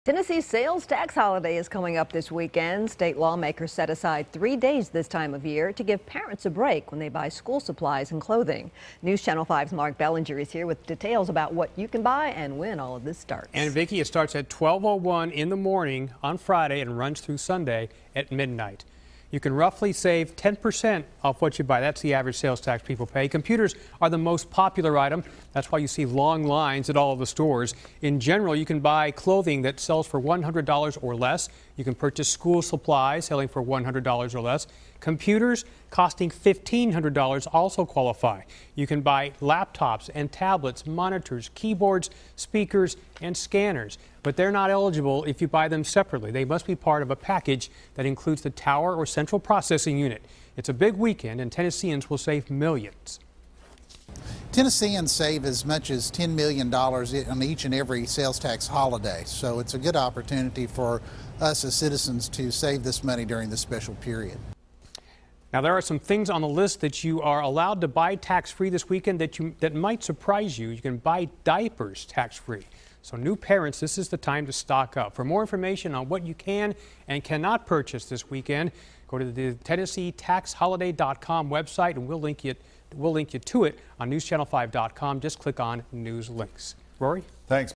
Tennessee Sales Tax Holiday Runs Friday Through Sunday
The state’s annual sales tax holiday will give Tennesseans a break as students head back to school. It starts at 12:01 a.m. Friday and ends Sunday at midnight.